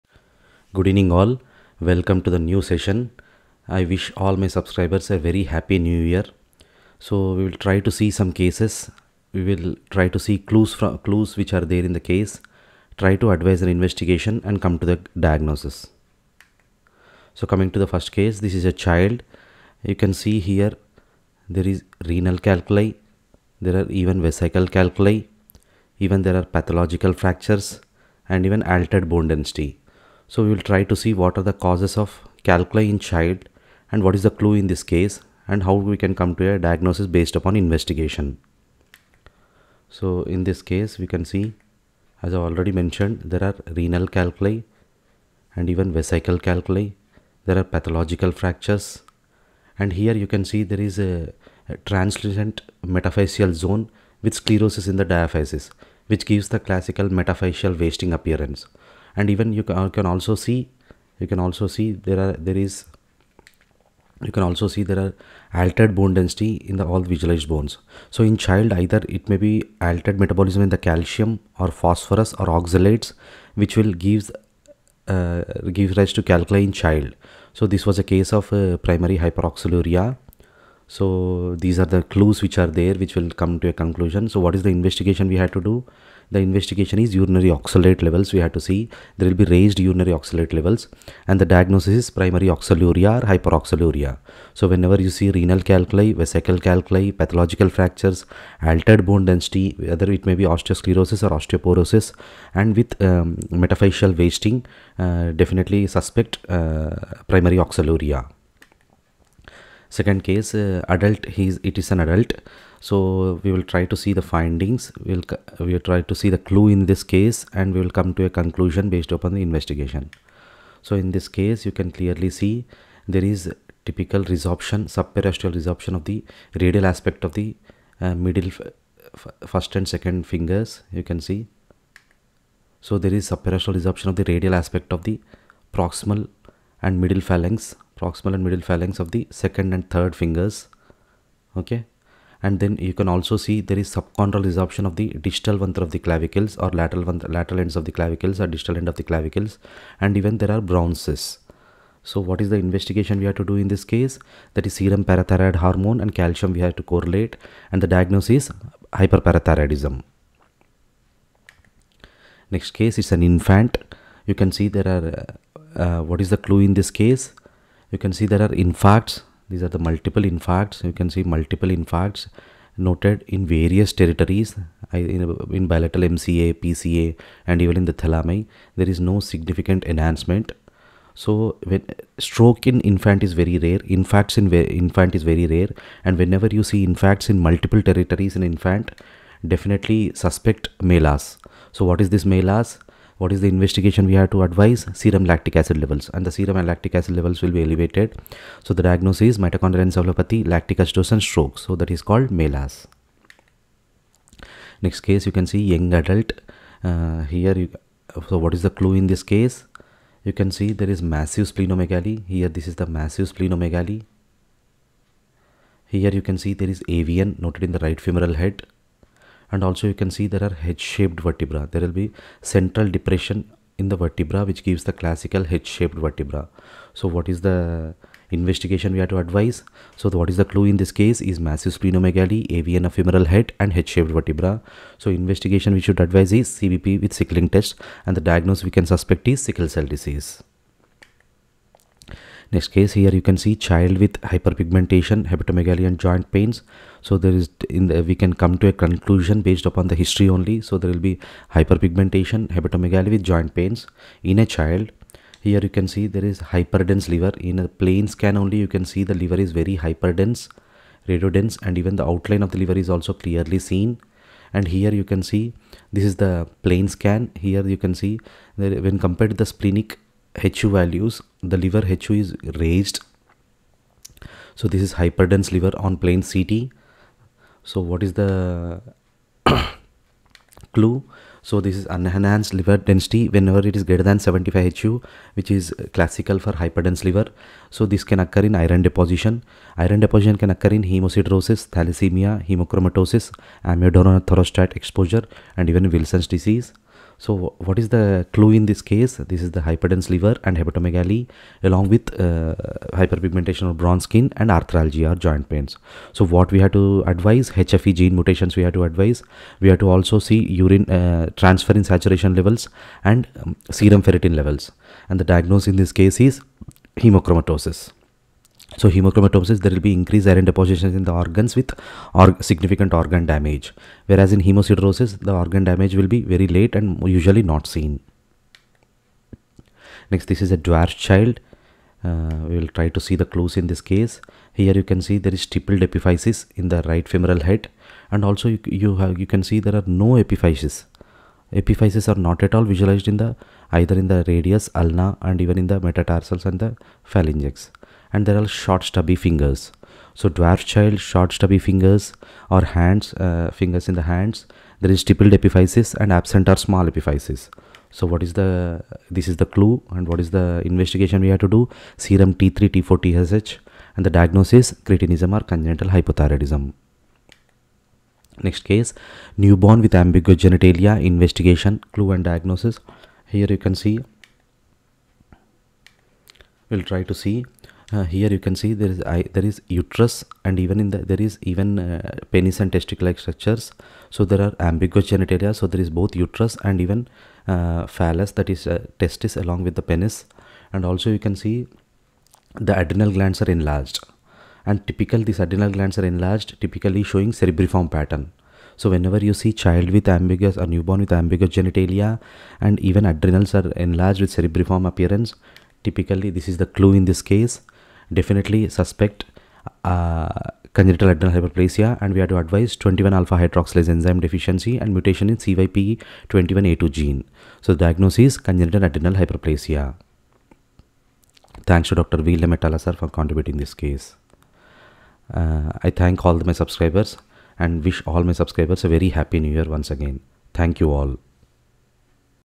Clue, Investigation and diagnosis (CID) series in Radiology
Radiology Cases with hidden clues and investigations to confirm the diagnosis

00:20 Primary Hyperoxaluria /oxalosis-Urine Oxalate levels.
01:50 Hyperparathyroidism - Serum PTH/Ca levels.
02:42 Mitochondrial encephalopathy, lactic acidosis and stroke(MELAS)-Serum Lactic acid levels.
03:24 Sickle cell disease -CBP/Sickling test.
04:04 Hemochromatosis - HFE gene mutations, Serum ferritin, Transferrin saturation levels.
05:30 Congenital hypothyroidism/Cretinism -Serum T3,T4 and TSH levels.
06:08 Congenital adrenal hyperplasia (CAH)- 21 -Alpha hydroxylase enzyme levels/CYP21A2 gene mutations.
07:08 Thank you all.